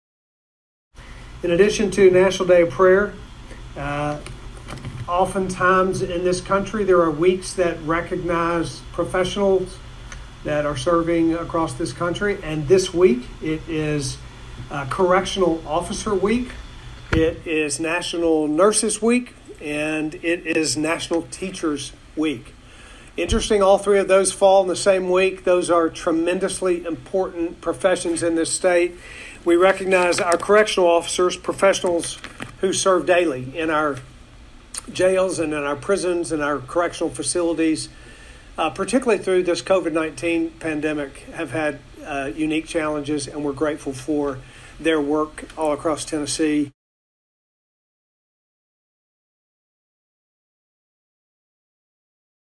Tennessee Governor Bill Lee on 2020
Thank you to Governor Bill Lee for recognition of CorrectionsWeek!